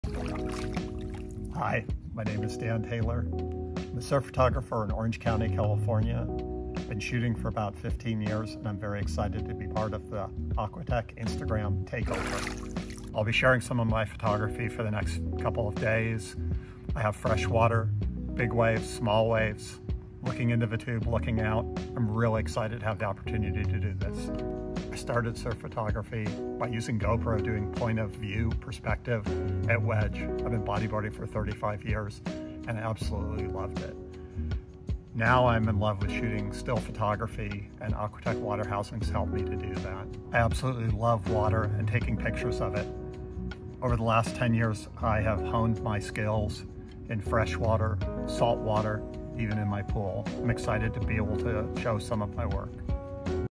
World Photography Day Takeover With Dan Taylor
August 19th is World Photography Day (WPD). A day to celebrate the joy of the photographic medium. A day that asks you to "share your world with the world". This year, we wanted to share the world of some special AquaTech users. Passionately devoted to their crafts and the moments they capture. Without the motivation of fame or fortune, they do it for the love and are constantly rewarded with the special moments that only water photographers can capture.

We're stoked to have our third and final creator Dan Taylor ( @danovisions ) close out our IG takeover series for worldphotographyday. Dan is an incredibly talented water photographer and shoots in a variety of conditions. Whether it be fresh water or salt water, heavy shorebreak or micro wedges he has a knack for catching aquatic oddities in all shapes and sizes. Dan embodies the spirit of the WPD takeover series because he shoots purely for his own enjoyment and love for the water. His account was actually private before we reached out to him for this series, he opened his profile to the public so all of you can check out his work. We encourage you all to do so and stick around for the posts to come!

Head to our IG page to check out Seb's work!